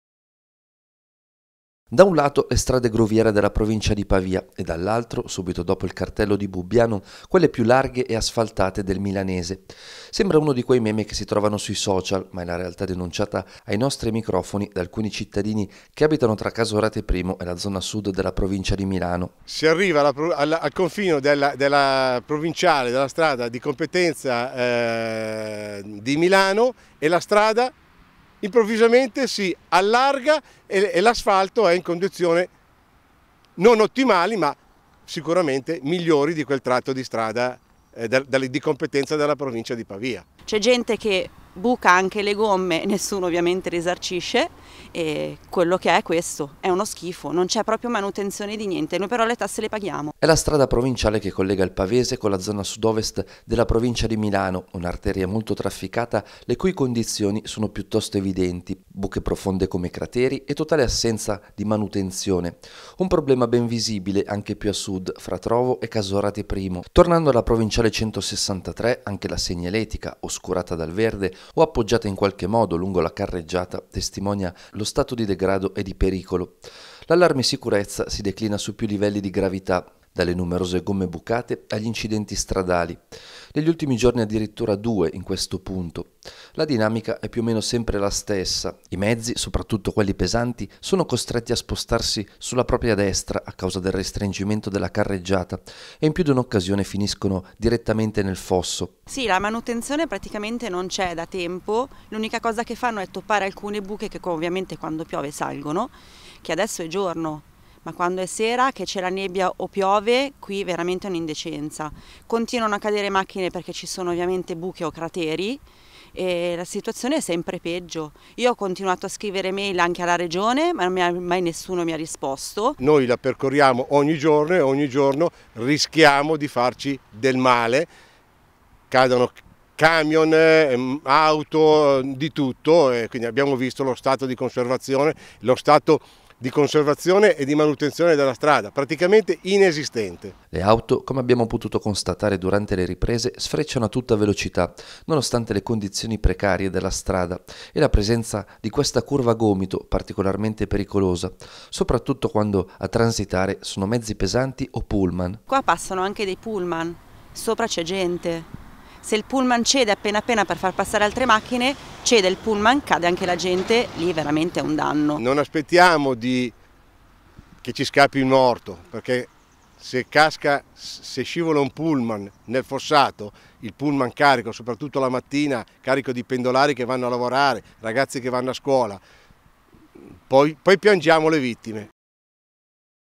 Strade provinciali colabrodo tra Pavia e Milano, i residenti: "Rischiamo la vita ogni giorno"
Da un lato le strade colabrodo della provincia di Pavia e dall’altro, subito dopo il cartello di Bubbiano, quelle più larghe e asfaltate del milanese. Sembra un di quei meme che si trovano sui social ma è la realtà denunciata ai nostri microfoni da alcuni cittadini che abitano tra Casorate Primo e la zona sud della provincia di Milano.

E’ la strada provinciale che collega il pavese con la zona sud ovest della provincia di Milano. Un’arteria molto trafficata le cui condizioni sono piuttosto evidenti. Buche profonde come crateri e totale assenza di manutenzione. Un problema ben visibile anche più a sud, fra Trovo e Casorate Primo.

Tornando alla provinciale 163, anche la segnaletica, oscurata dal verde o appoggiata in qualche modo lungo la carreggiata, testimonia lo stato di degrado e di pericolo. L’allarme sicurezza si declina su più livelli di gravità: dalle numerose gomme bucate agli incidenti.

Negli ultimi giorni addirittura due nello stesso punto. La dinamica è quasi sempre la stessa: i mezzi, soprattutto quelli pesanti, sono costretti a spostarsi sulla propria destra, a causa del restringimento della carreggiata, e in più di un’occasione finiscono direttamente nel fosso.

Le auto, come abbiamo potuto constatare durante le riprese, sfrecciano a tutta velocità nonostante le condizioni precarie della strada e la presenza di questa curva a gomito particolarmente pericolosa, soprattutto quando a transitare sono mezzi pesanti o pullman.